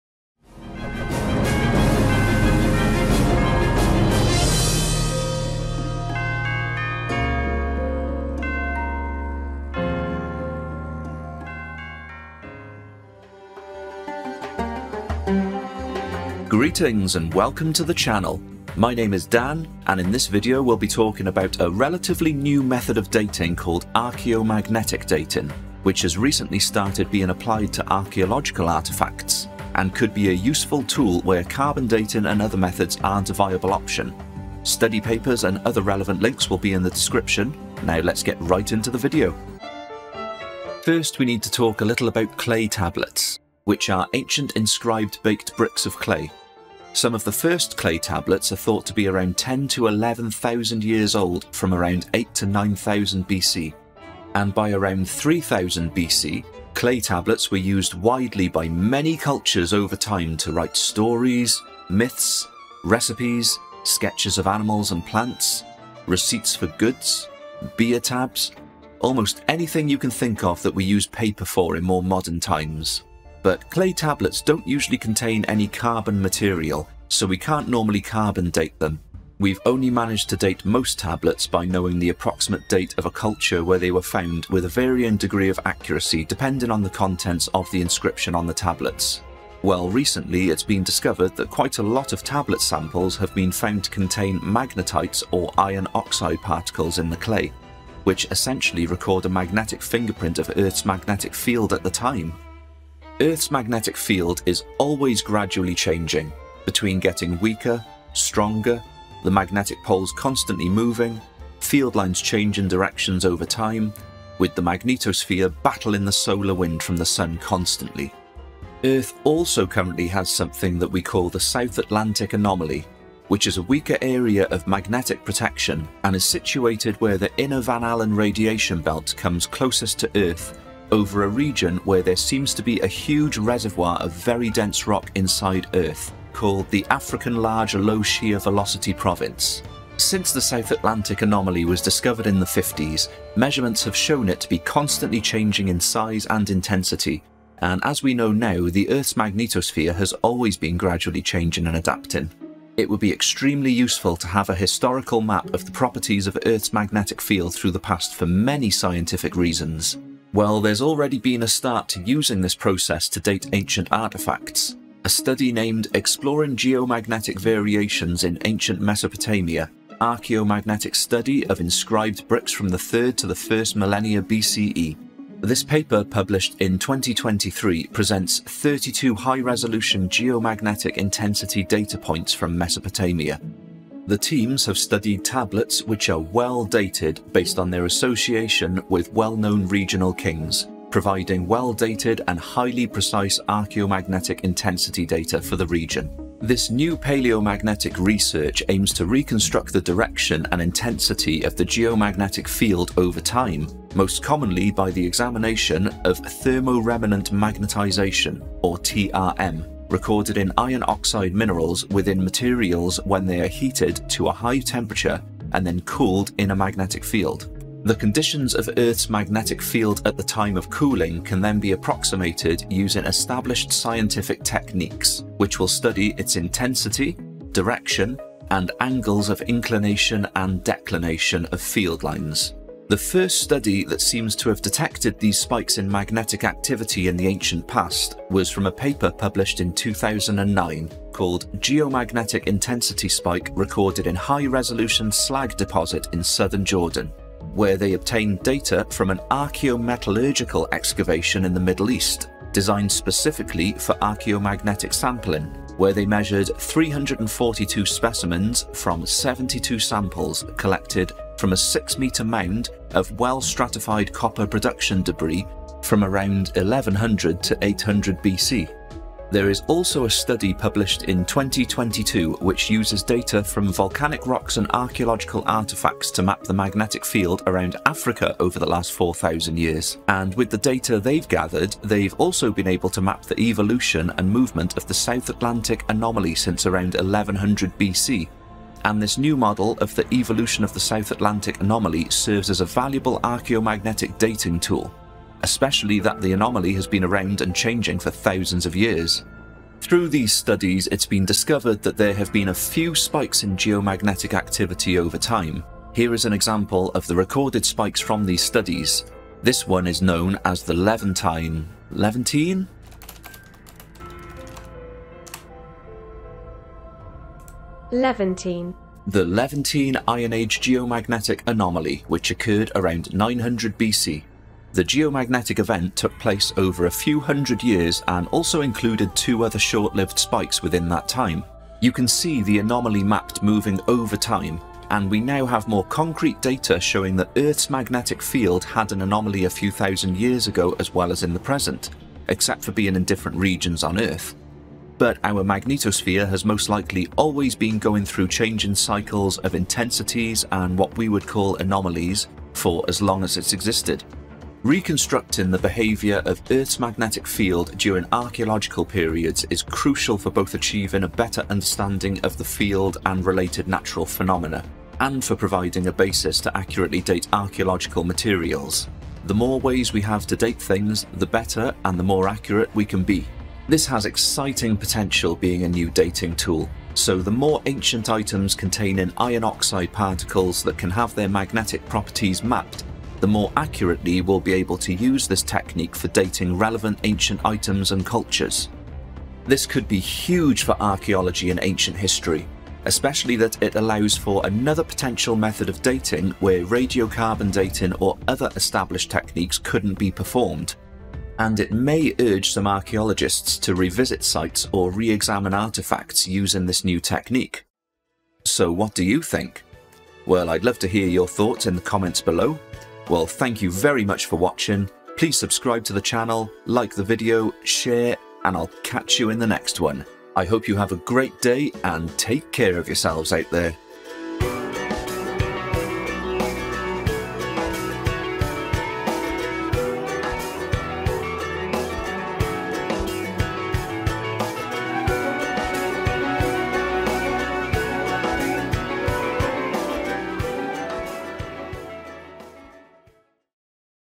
NEW DATING Method Mapping Earth's Magnetic Field Through Time!
This video discusses how we are starting to map Earth's magnetic field through the past using a technique called "Archaeomagnetic Dating", and how we can then use those maps to date ancient artifacts which have been heated and cooled to form, like clay tablets, specifically ones with iron oxide particles that would record a magnetic fingerprint at the time of being formed.

Papers:
Exploring geomagnetic variations in ancient Mesopotamia: Archaeomagnetic study of inscribed bricks from the 3rd–1st millennia BCE

Geomagnetic intensity spike recorded in high resolution slag deposit in Southern Jordan

A first regional model of the past Earth's magnetic field from Africa for the last 4000 years